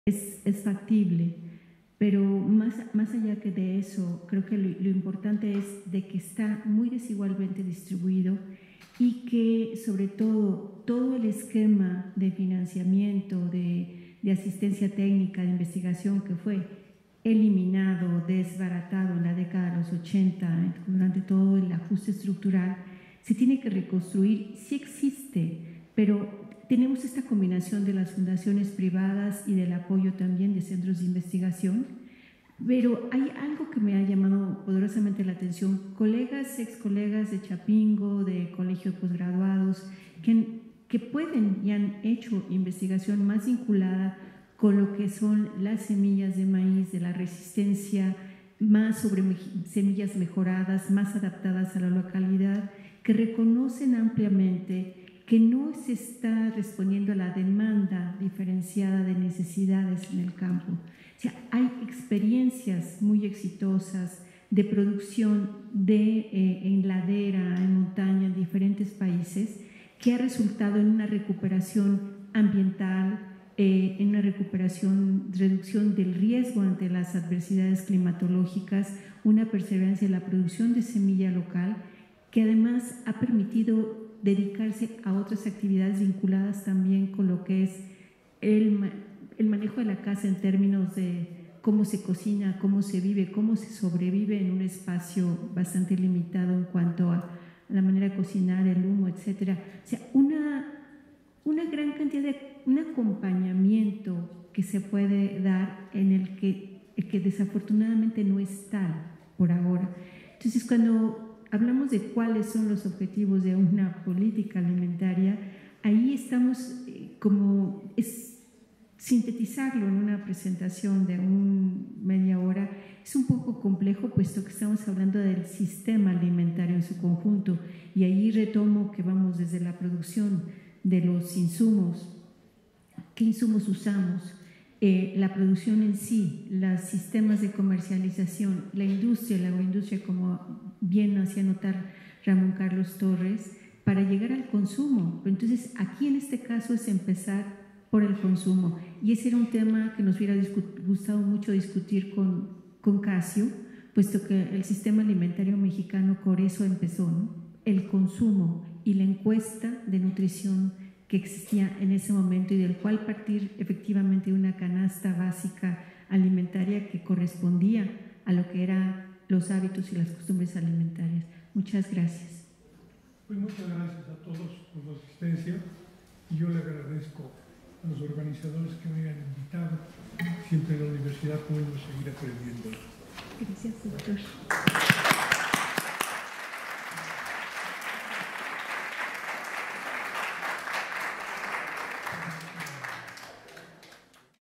6º Diálogo Nacional por un México Social: Regresar a lo fundamental, Mesa 5, 5/5
Mesa 5 "Alimentación"
Ponente: Cassio Luiselli, Instituto Tecnológico y de Estudios Superiores de Monterrey
Comentan: Gustavo Gordillo, investigador independiente y Margarita Flores, Programa Universitario de Estudios del Desarrollo, UNAM
Modera: Octavio Rivero Serrano, ex rector de la UNAM